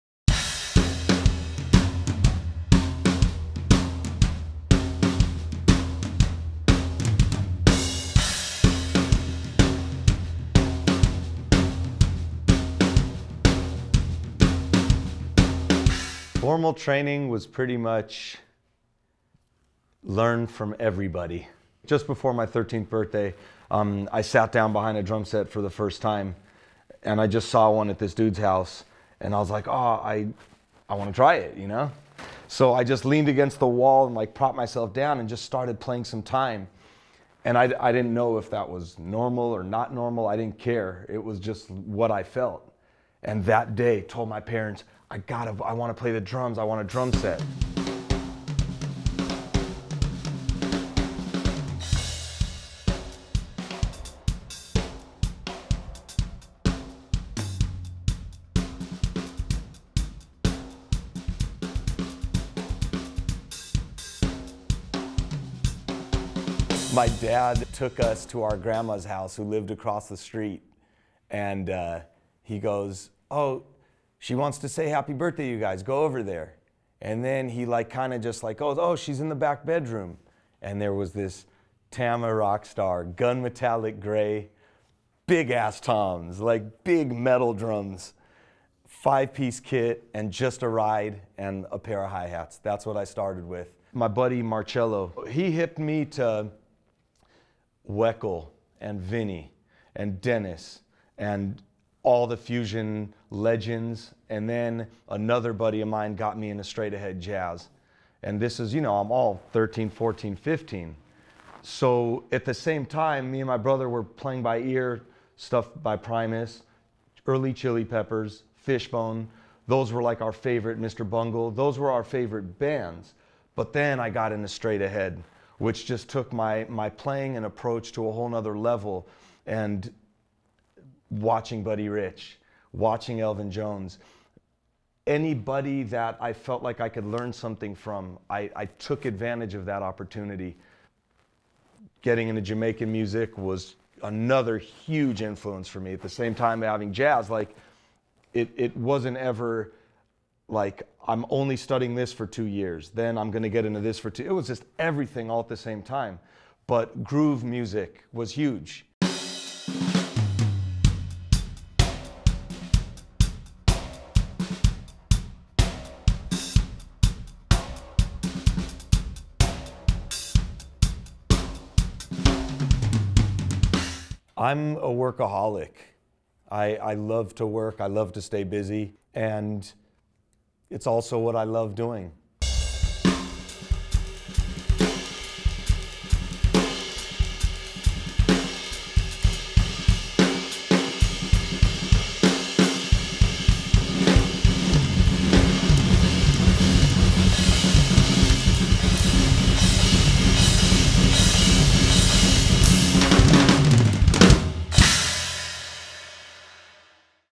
Gil Sharone Interview
Gil discusses his drummer past, unique styles and what drums mean to him.